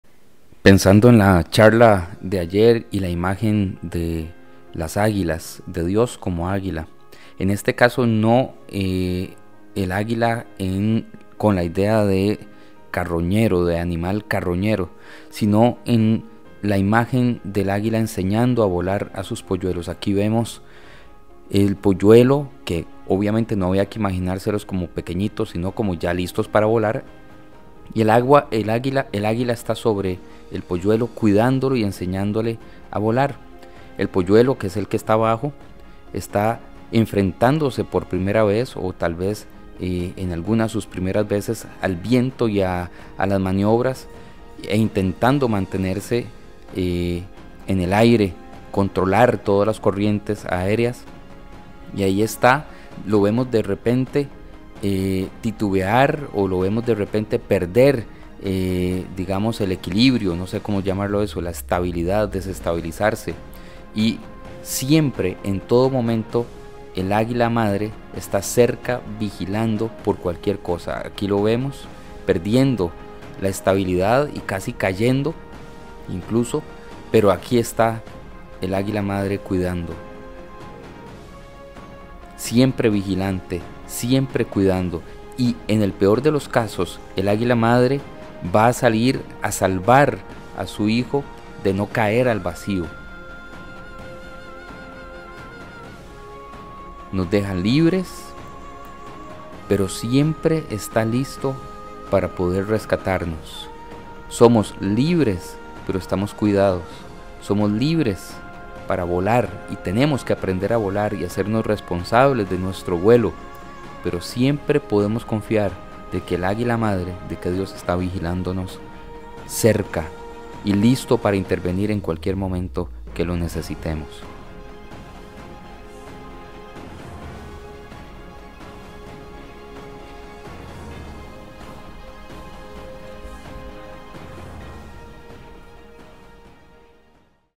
El águila enseña a volar a su polluelo
Un pequeño comentario sobre la imagen de Dios como águila expresada en el Éxodo.